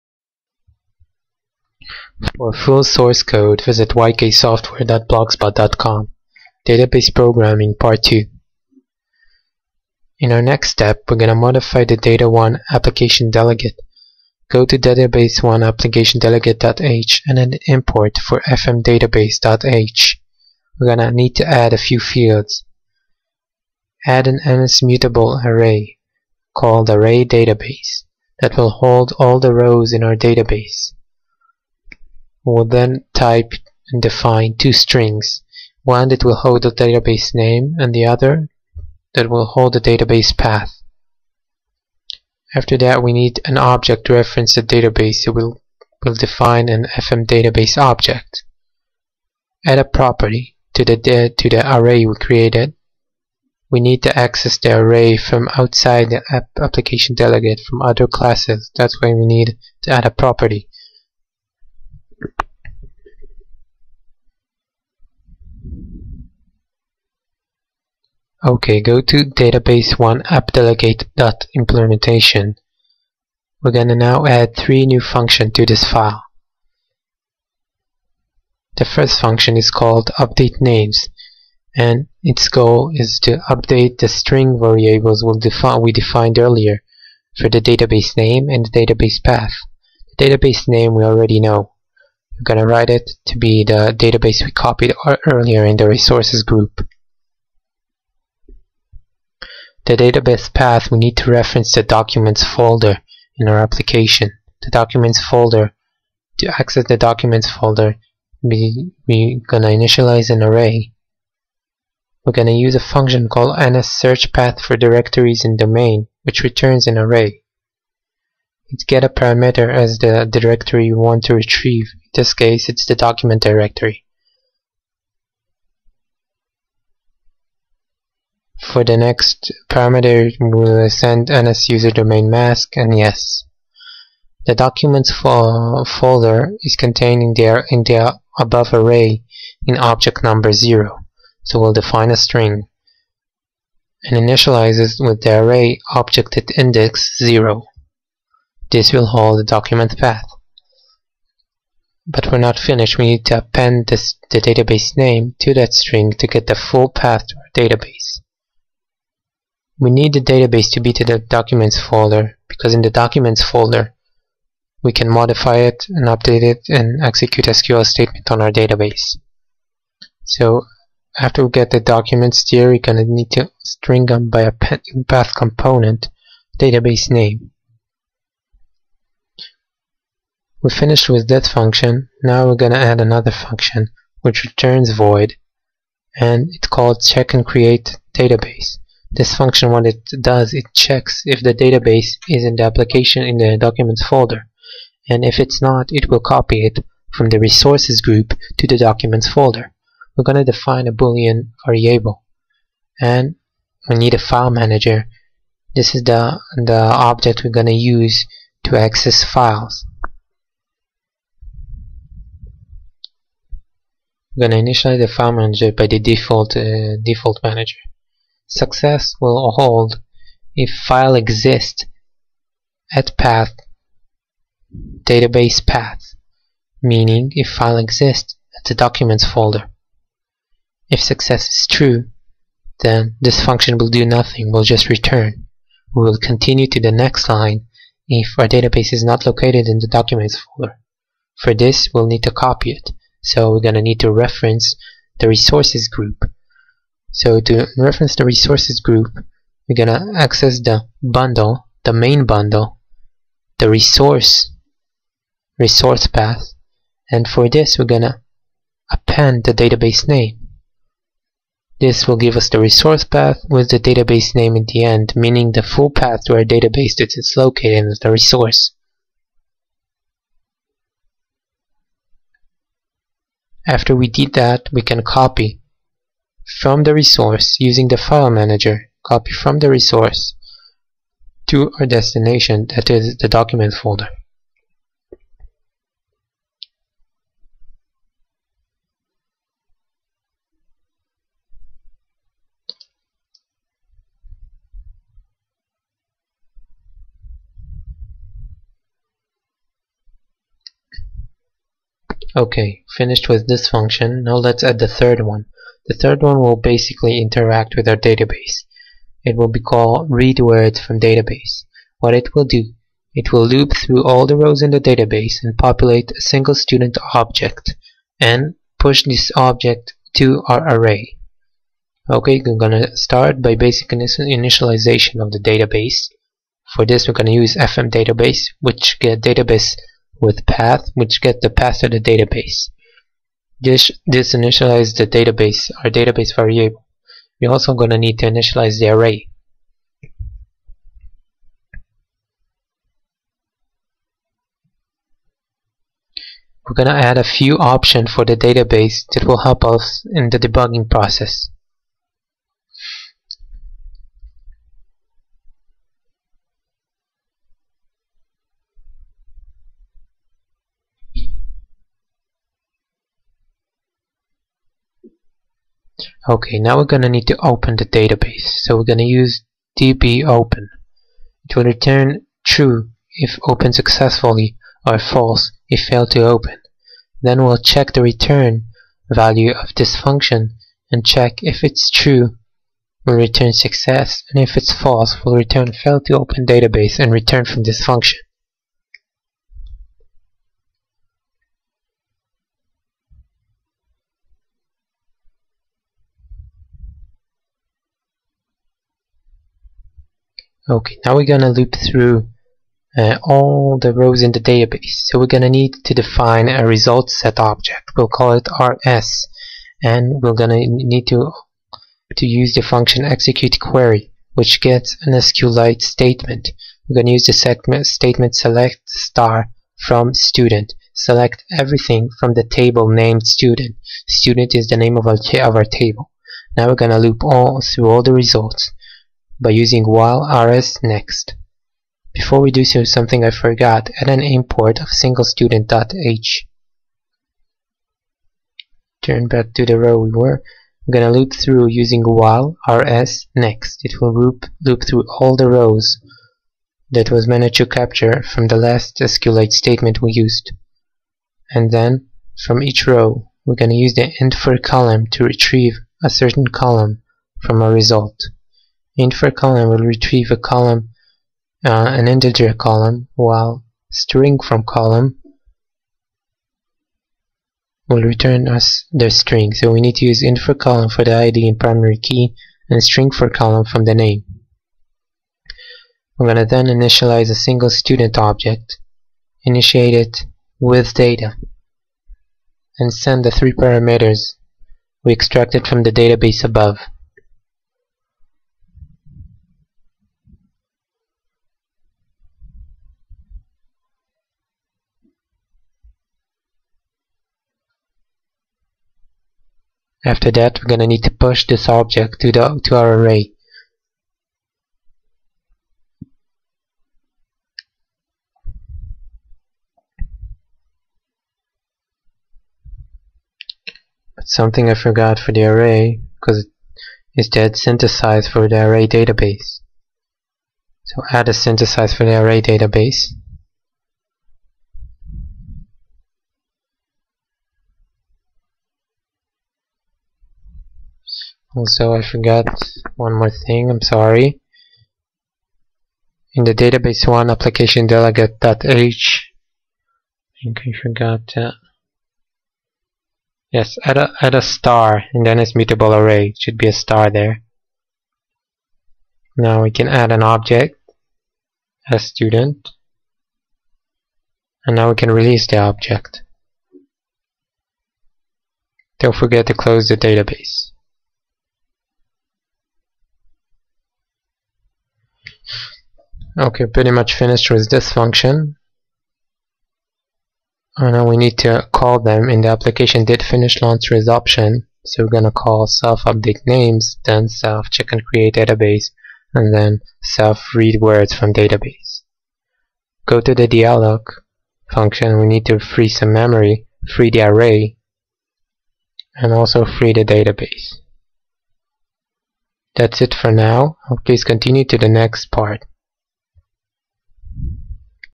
iPhone tutorial database programming part2
In this video u will learn how to add a database to your application, we will cover the basics of database programming, learn about sqlite and sqlite wrapper class for easy interaction with the database.
we will create a program which loads data from a database to a table.